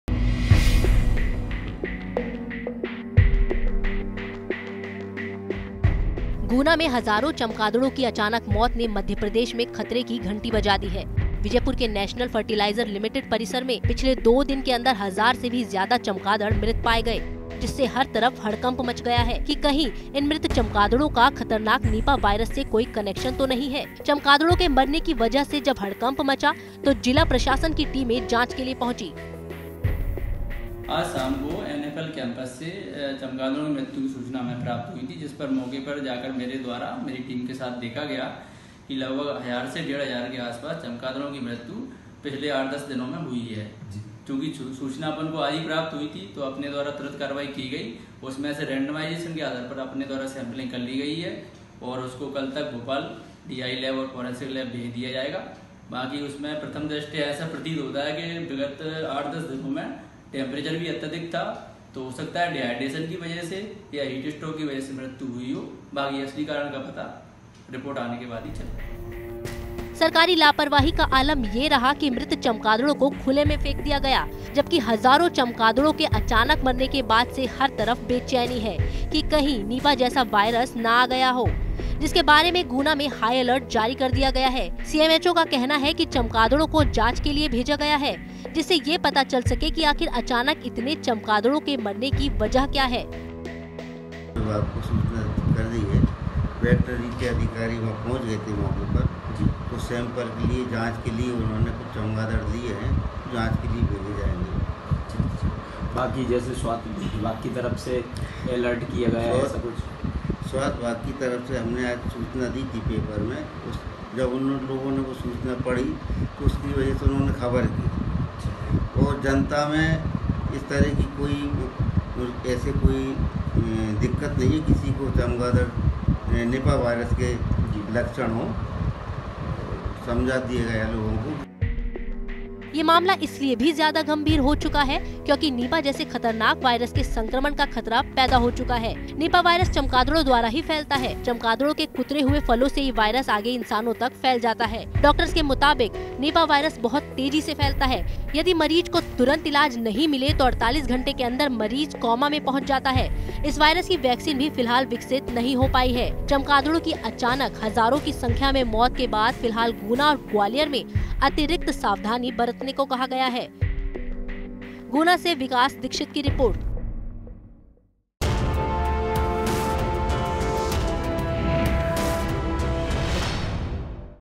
मध्यप्रदेश के इन दो जिलों में मंडराया nipah virus का खतरा, हाई अलर्ट जारी ! |MPTak
मध्यप्रदेश के गुना  में नेशनल फर्टिलाइज़र लिमिटेड परिसर अचानक हजारों की संख्या में चमगादड़ों की मौत के बाद इलाके बाद  nipah virus का खतरा मंडराने लगा है... इसे लेकर प्रशासन ने हाई अलर्ट जारी किया है..